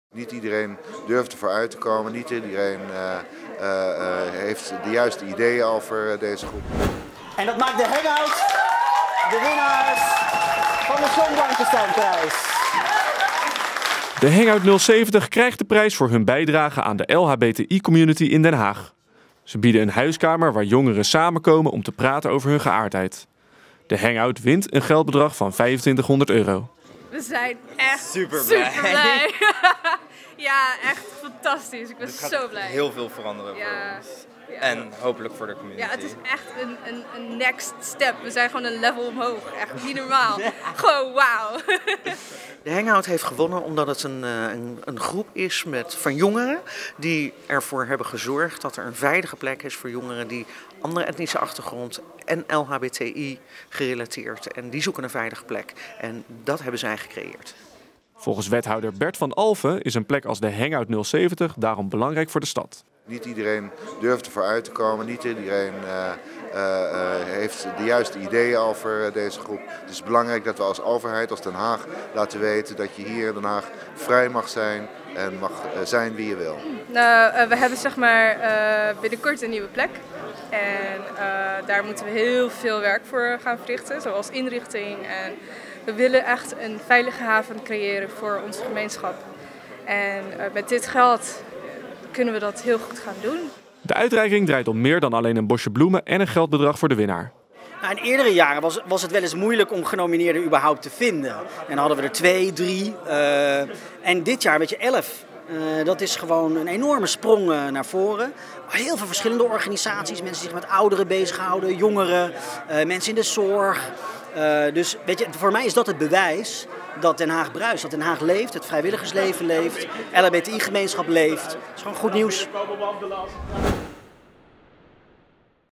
The Hang-Out 070 wint John Blankensteinprijs
The Hang-Out 070 heeft dinsdagavond in Theater Dakota de John Blankensteinprijs in ontvangst mogen nemen. Het initiatief biedt een huiskamer waar jongeren samenkomen om te praten over seksualiteit, genderidentiteit, liefde en relaties. Daarnaast organiseren ze activiteiten voor LHBTQ- jongeren.